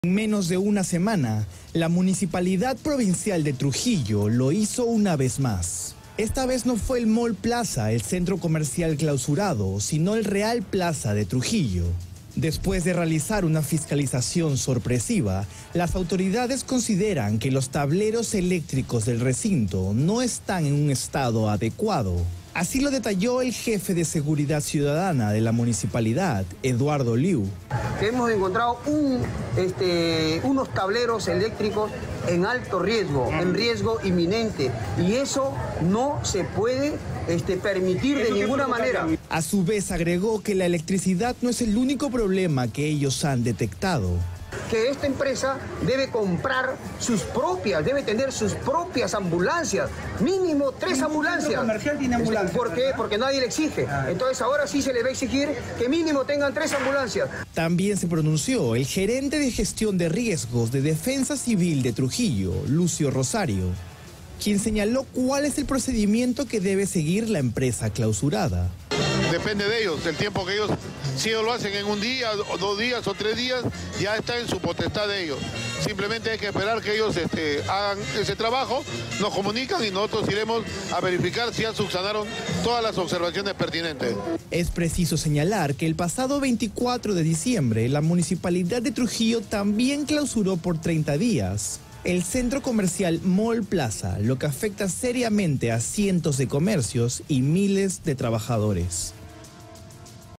Cierran otro centro comercial en Trujillo: ¿Por qué la municipalidad clausuró conocido recinto?
La Municipalidad Provincial de Trujillo clausuró el Real Plaza de Trujillo después de realizar una fiscalización sorpresiva, donde consideraron que los tableros eléctricos del recinto no estaban en un estado adecuado.

Eduardo Liu, jefe de Seguridad Ciudadana de la municipalidad, informó que los tableros eléctricos del centro comercial presentan un riesgo inminente y que esta situación no puede ser permitida bajo ninguna circunstancia. Además de los problemas eléctricos, se indicó que la empresa debe contar con al menos tres ambulancias, ya que hasta el momento no se les exigía este requisito.

Por su parte, Lucio Rosario, gerente de Gestión de Riesgos de Defensa Civil de Trujillo, indicó que el procedimiento para levantar la clausura dependerá de la empresa. Esta deberá solucionar los problemas encontrados y comunicar a las autoridades para que verifiquen las correcciones realizadas.

Cabe recordar que el pasado 24 de diciembre, la municipio clausuró por 30 días el Mall Plaza luego que se desarrollara una balacera en sus interiores, lo que ha afectado a numerosos comercios y miles de trabajadores.